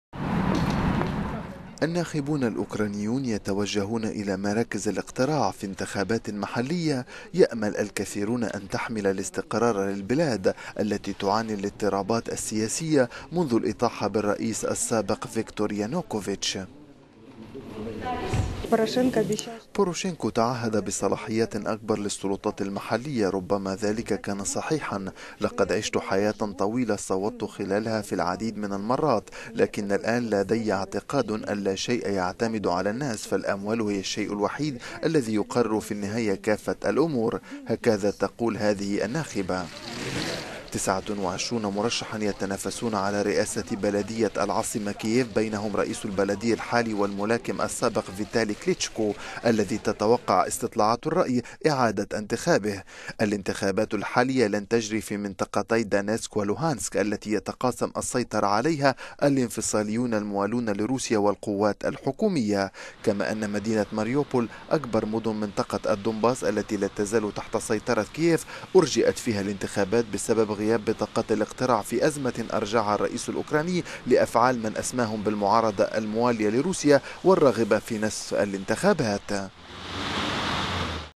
انتخابات محلية في أوكرانيا دون منطقة الدونباس
الناخبون الأوكرانيون يتوجهون إلى مراكز الاقتراع في انتخابات محلية يأمل الكثيرون أن تحمل الاستقرار للبلاد التي تعاني الاضطرابات السياسية منذ الاطاحة بالرئيس السابق فيكتور يانوكوفيتش.

ناخبة أوكرانية:
"بوروشينكو تعهد بصلاحيات أكبر للسلطات المحلية ربما ذلك كان صحيحا. لقد عشت حياة طويلة وصوت خلالها في العديد من المرات. لكن الان لدي اعتقاد ان لا  شيء يعتمد على الناس. فالأموال هي الشيء الوحيد الذي يقرر في النهاية كافة الأمور."

   29 مرشحا يتنافسون على رئاسة بلدية العاصمة كييف بينهم رئيس البلدية ال…